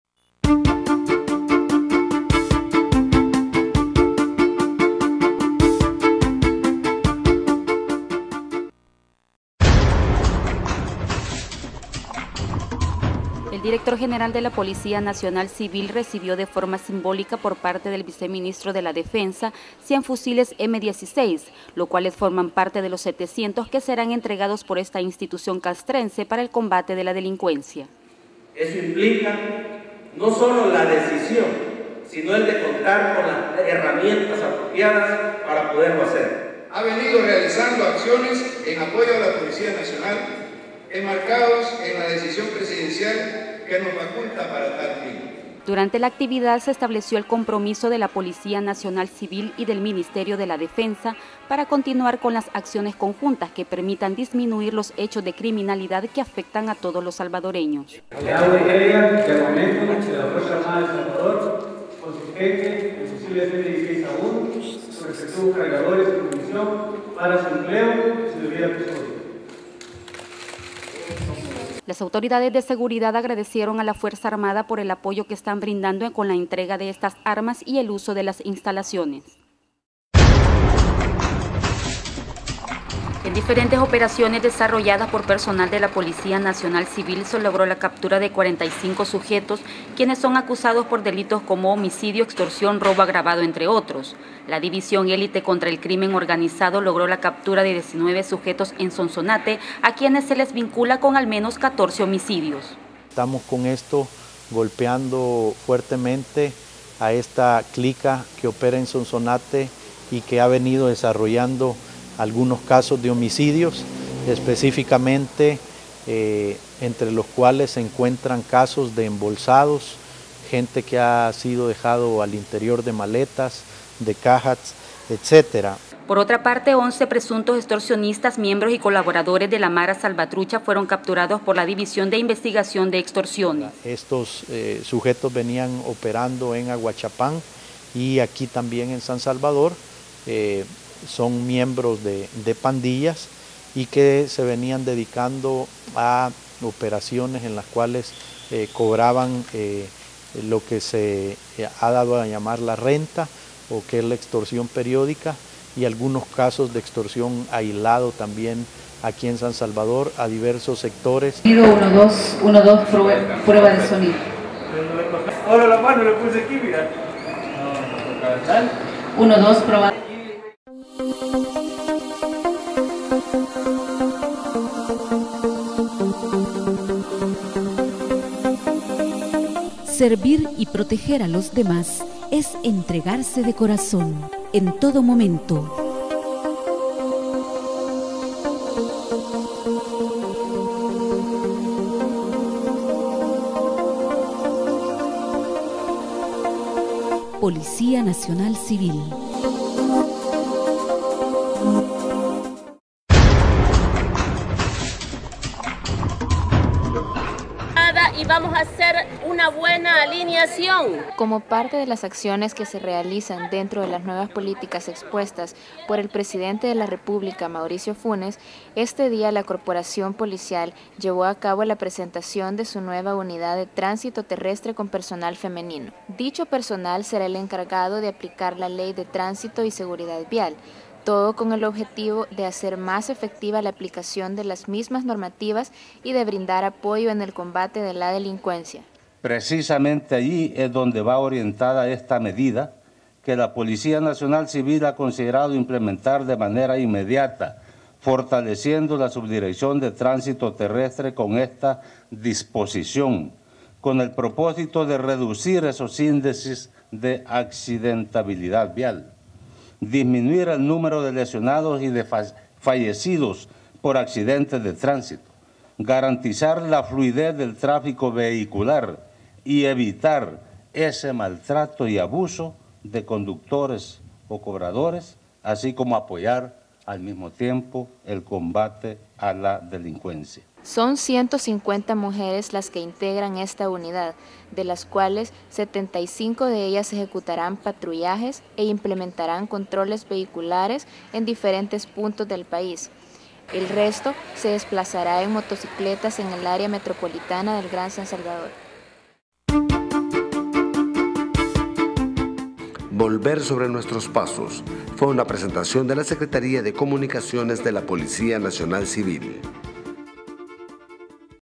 VSNP 22 FEB 2010.mpg
-45 DETENIDOS EN DIFERENTES OPERATIVOS
-DESPLIEGUE 150 MUJERES DE TRANSITO